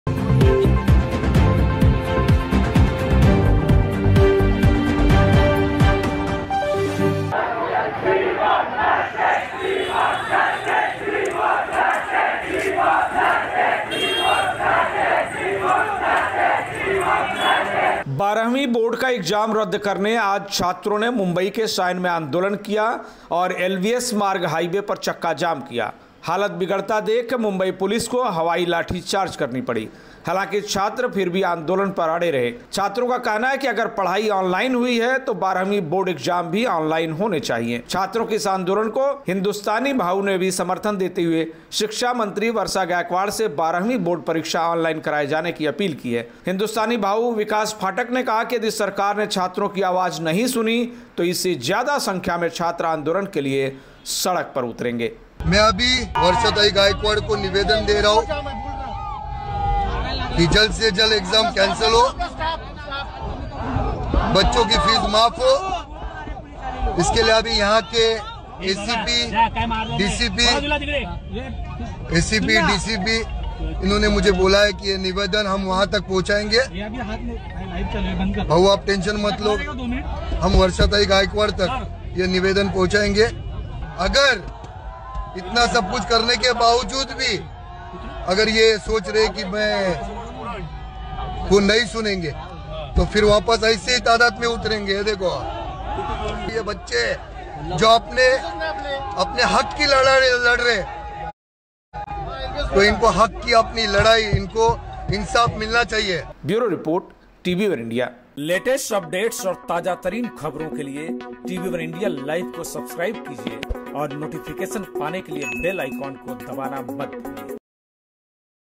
Hindustani Bhau arrested : छात्रों को समर्थन देने पर कार्रवाई | Students opposing Offline Exams
HindustaniBhauArrested : छात्रों को समर्थन देने पर कार्रवाई
बोर्ड एक्जाम रद्द कराने छात्रों ने रोका मुंबई हाईवे || hindustani bhau cancel board exam 2022
2वीं बोर्ड का एक्जाम रद्द करने आज छात्रों ने मुंबई के सायन में आंदोलन किया और एलबीएस मार्ग हाईवे पर चक्काजाम किया. हालत बिगड़ता देख मुंबई पुलिस को हवाई लाठी चार्ज करनी पड़ी, हालांकि छात्र फिर भी आंदोलन पर अड़े रहे. छात्रों का कहना है कि अगर पढ़ाई ऑनलाईन हुई है तो बोर्ड एक्जाम भी आनलाईन होने चाहिए. छात्रों के इस आंदोलन को हिन्दुस्तानी भाऊ ने समर्थन देते हुए शिक्षा मंत्री वर्षा गायकवाड़ से 12वीं बोर्ड परीक्षा आनलाईन कराए जाने की अपील की है. हिन्दुस्तानी भाऊ विकास फाटक ने कहा कि यदि सरकार ने छात्रों की आवाज नहीं सुनी तो इससे ज्यादा संख्या में छात्र आंदोलन के लिए उतरेंगे.